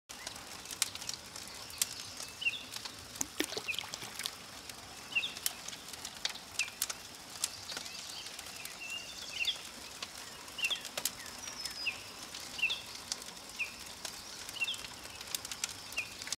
Satake Outdoor - Dutch Oven hot Chocolate
Dutch Oven, Eldkrok och Tripod-XL - varm choklad i det fria - Satake Outdoor.